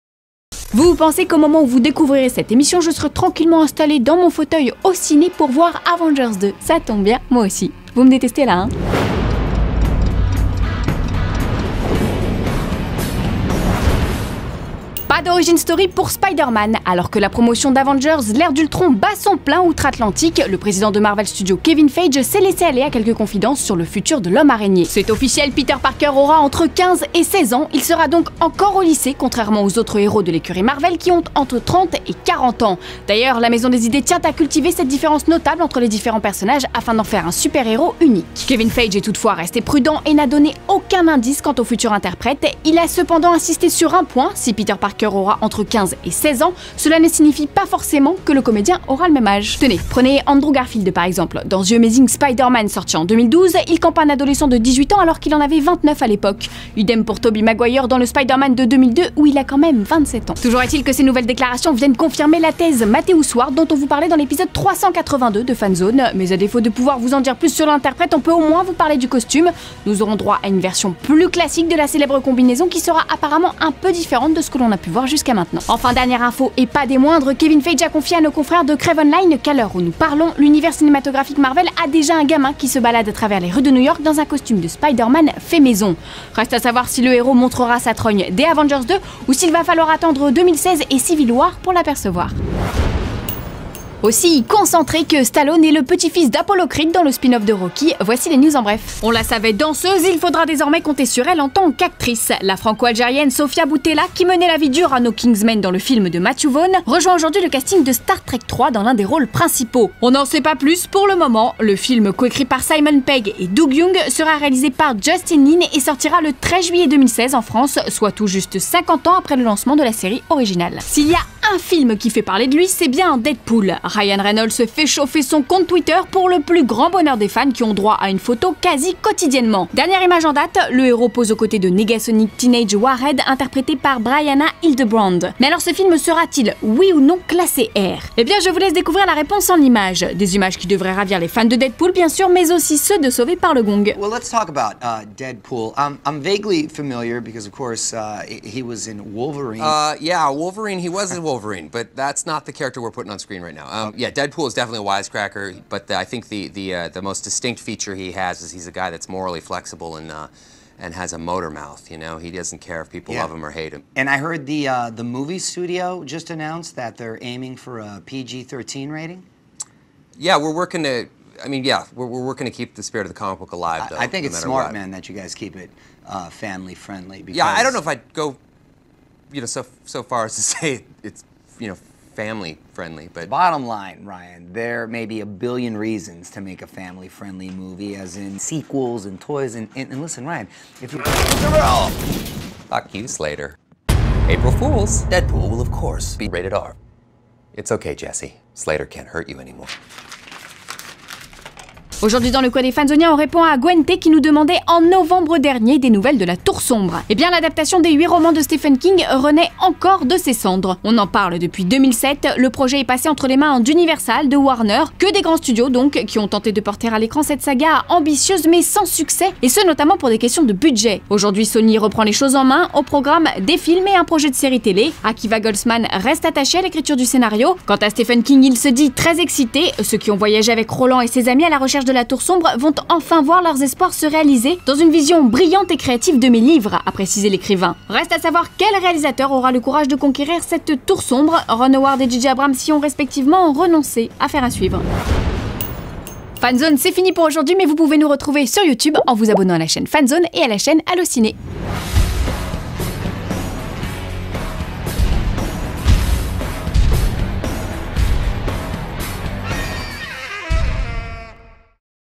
Les dernières infos sur Spider-Man
Quelques infos sur le futur Spider-Man, Sofia Boutella dans Star Trek 3, Ryan Reynolds continue la promo pour Deadpool...